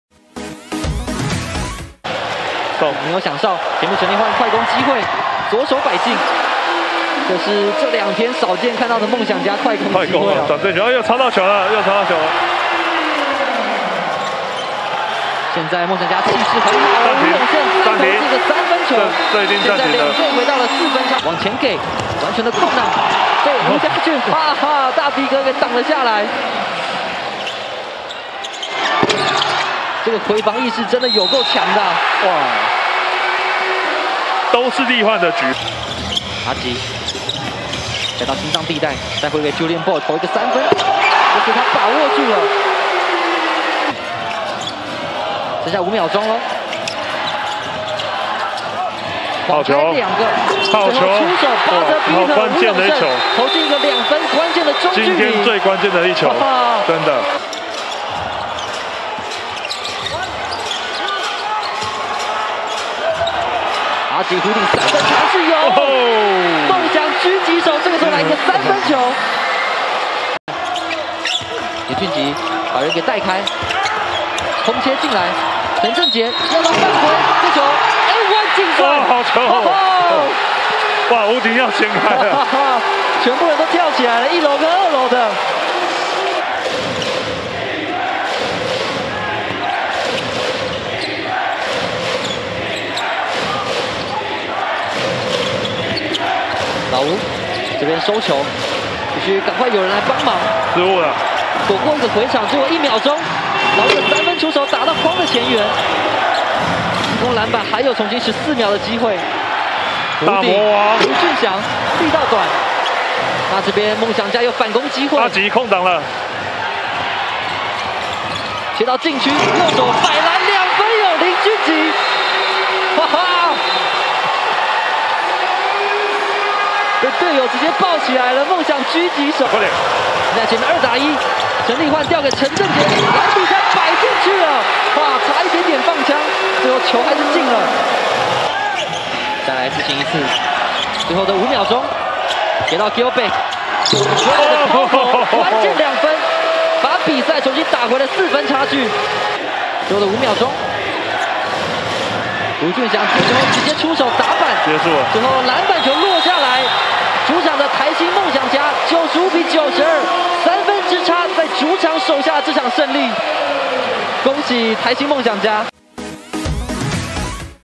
夢想家苦勝領航猿高潮迭起 啦啦隊直接看球不應援了
台新夢想家加時賽苦勝桃園領航猿~扣人心弦!